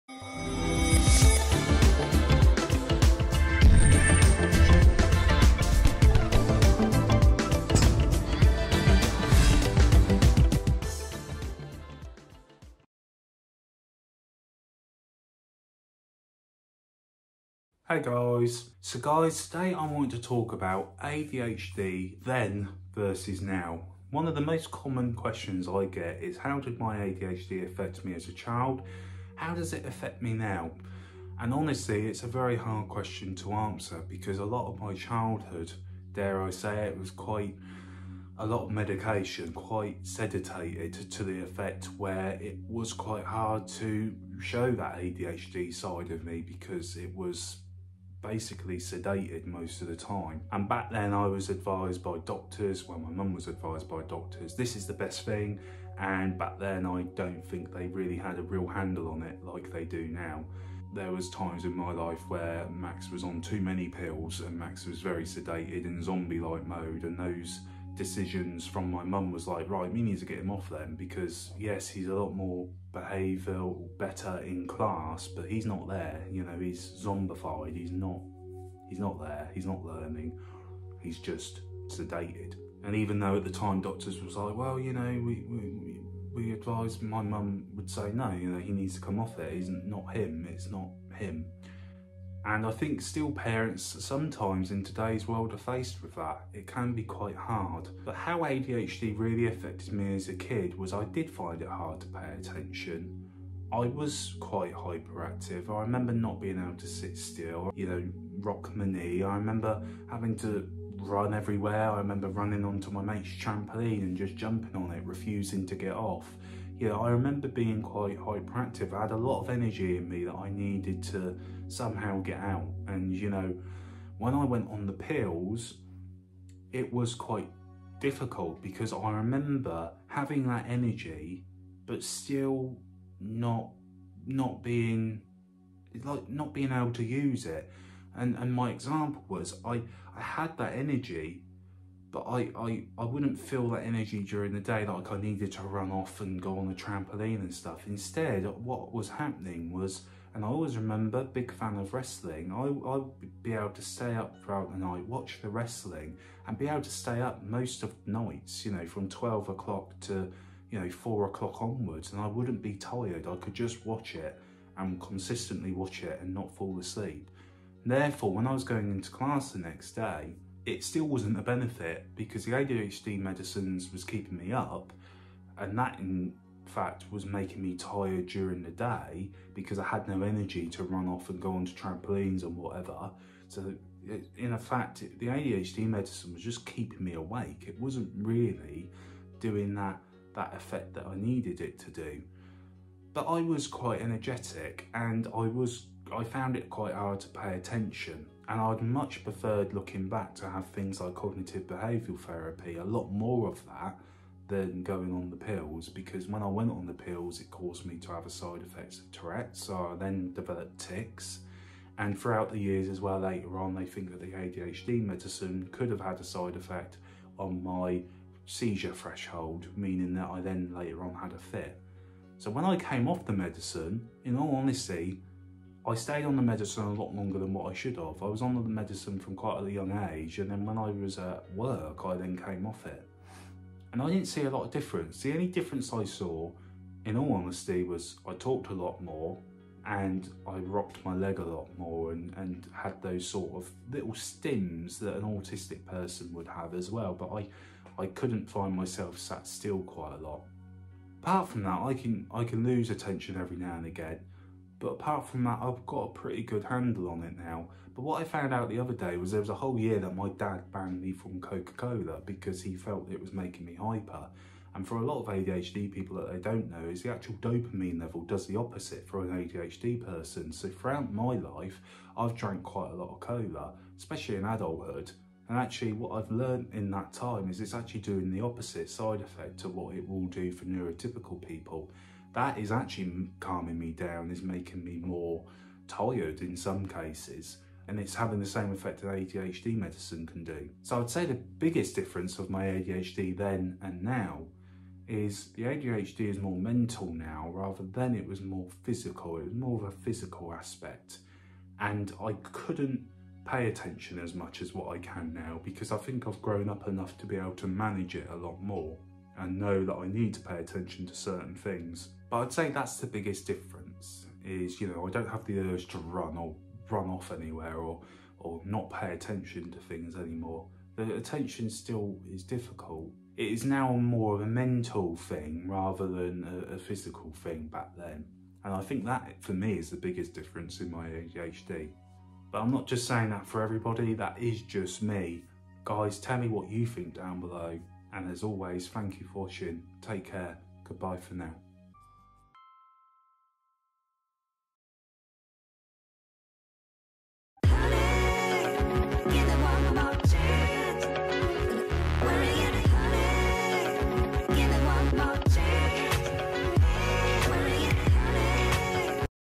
ADHD Then Vs Now | MaxiAspie
In This Video I Talk About How ADHD Affected Me Then Vs Now, I talk about how it affected me as a child and how it affects me as an Adult.

"Physical Now Mental"

My Links!